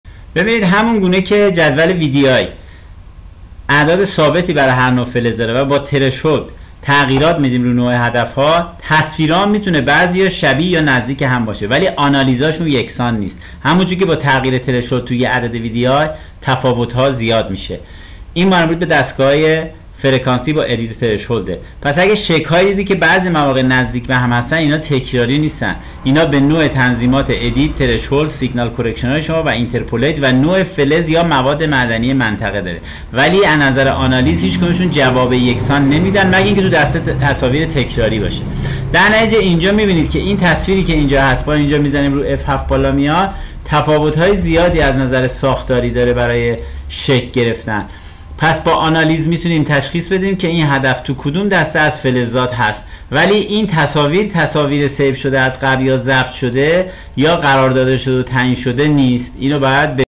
آنالیز لکه رنگ در فلزیاب تصویری
انواع لکه رنگ در فلزیاب تصویری که طبق اصول رادار تصویری مدار ان طراحی شده باشد آشکار میگردد و تشخیص شرایط نوع فلزات یا طلا یا منابع دیگر به مهارت و نوع انالیز یا مهارت اپراتور بستگی دارد و اپراتور فلزیاب تصویری باید بداند که با هر نوع تصویر و لکه رنگ های متفاوت چگونه روش تنظیمات در مدار اصلی فلزیاب تصویری مانند ادیت EDIT با عدد وی دی ای VDI و تنظیمات سطح و حجم ترشهولد THRESHOLD و برنامه نرم افزار فلزیاب تصویری تعیین و تغییر داده تا بتواند وجود هدف اصلی یا طلا یا فلز مورد نظر را تشخیص داده و مشخص نماید